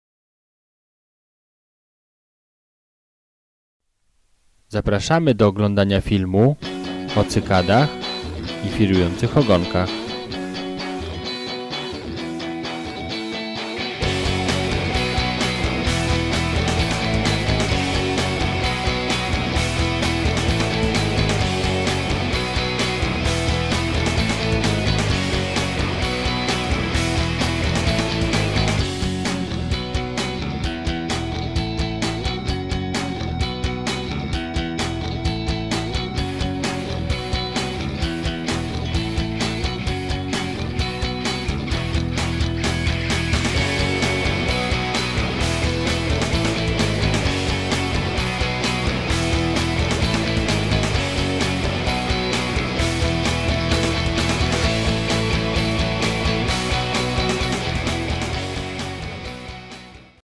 WĘDKARSTWO - SPINMAD - Cykady i Wirujące Ogonki
Wstęp do filmu, na którym firma Spinmad (producent cykad i wirujących ogonków) wraz z przewodnicywedkarscy.pl prezentują łowienie pstrągów potokowych na spinning. W trakcie kręcenia materiału filmowego, przewodnik Sebastian Kalkowski oraz właściciele Spinmad łowią pstrągi wykorzystując przynęty Spinmad. Film został nakręcony na jednej z pomorskich rzek pstrągowych. Podczas kręcenia materiału zostało złowionych kilka ładnych pstrągów, w tym okaz ponad 50cio centymetrowy. Na filmie możecie zobaczyć w akcji właścicieli Spinmad i przewodników. Prezentujemy sposoby łowienia przynętami Spinmad. Pokazujemy i omawiamy skuteczne techniki prowadzenia tych przynęt. Zapraszamy do obejrzenia trailera filmu! (Serdeczne podziękowania dla Artura i Huberta Mikulskich za wkład w montażu filmu!)